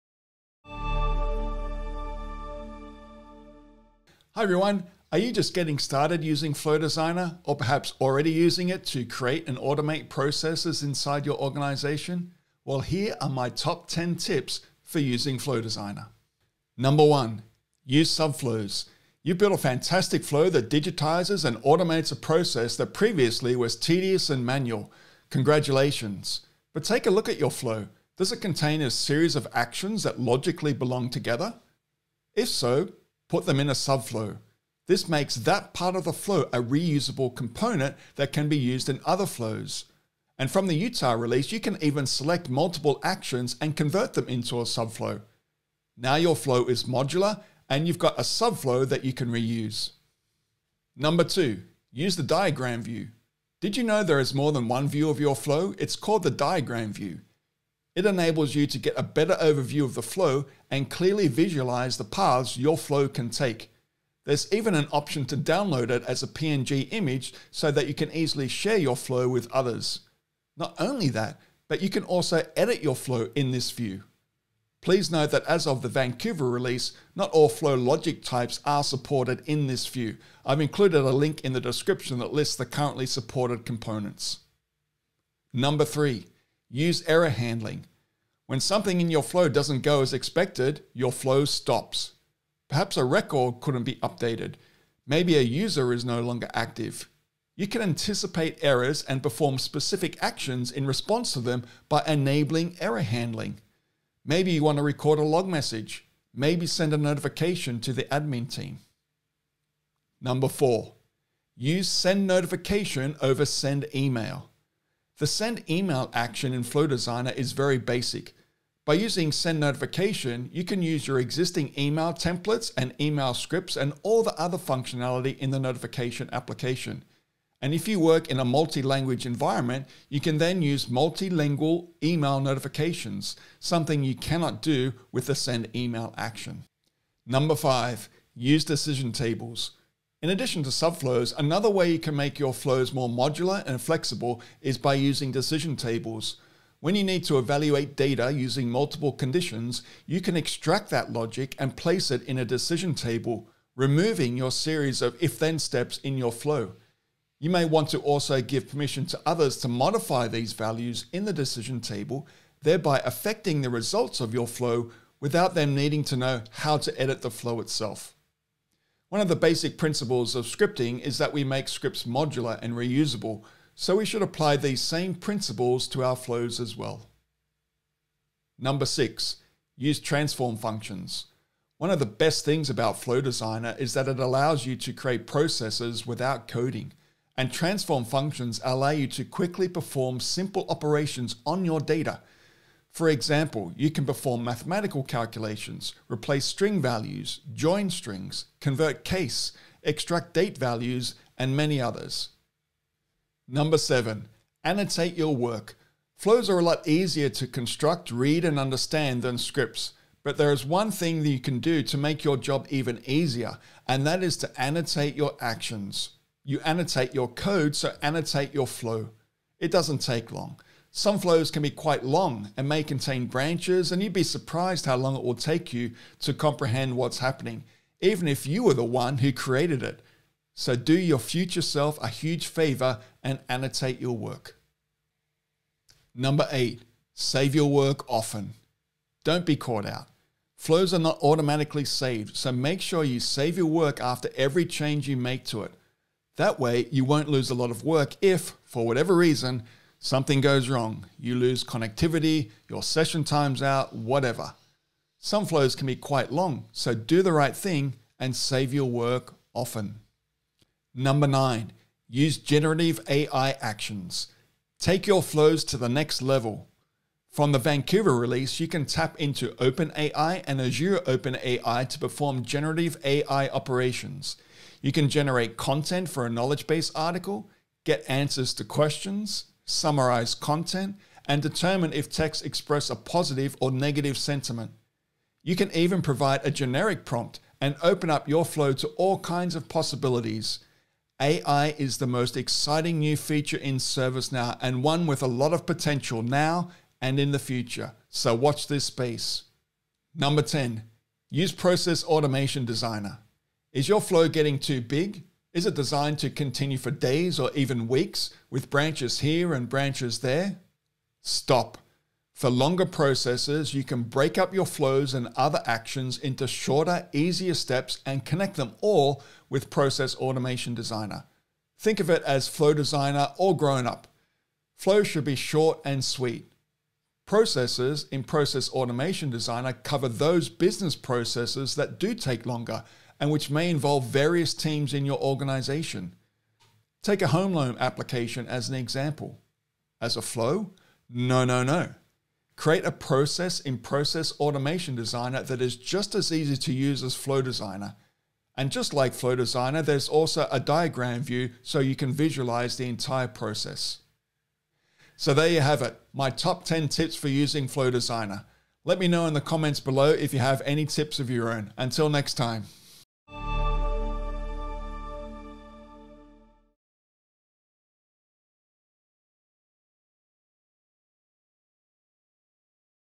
ServiceNow – Top 10 Tips for Using Flow Designer
Here are my Top 10 Tips for Flow Designer, ranging from original features to a brand new one in the Vancouver release. There is even a tip to *not* use Flow Designer! Can you guess what it is?

Further learning:

1. Flow Designer: Create Subflows and Actions (Now Learning course)

2. Diagram View (Documentation) (includes list of supported flow components)

3. Error handling (Documentation)

4. Send Notification (Documentation)

5. Simplify app development with Decision Builder (Knowledge 23 lab)

6. Transform functions (Documentation)

7. Annotations
8. Saving your work

9. Generative AI Controller Esssentials (Now Learning course)

10. Introduction to Playbooks and Process Automation Designer (Now Learning course)